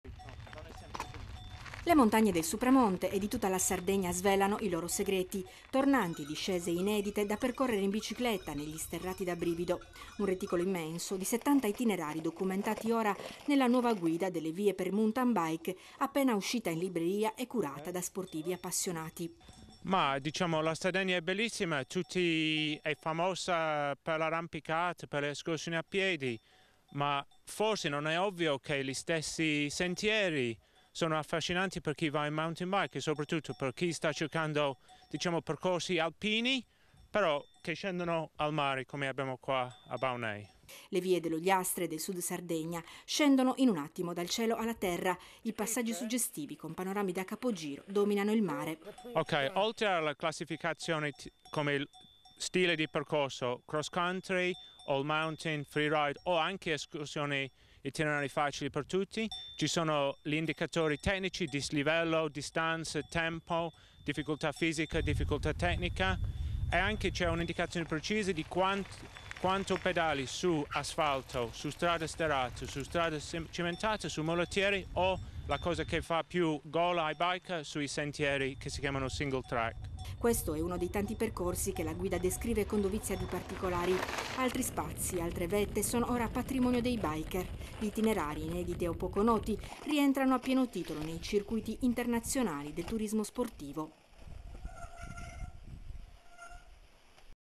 MOUNTAIN BIKE GUIDA VIDEOLINA
Servizio Videolina giovedì 29 dicembre 2011 sulla guida "Mountain Bike in Sardegna". Videolina (one of the two Sardinia region TV channels) transmission 29 December 2011 on new guide "Mountain Bike Sardinia"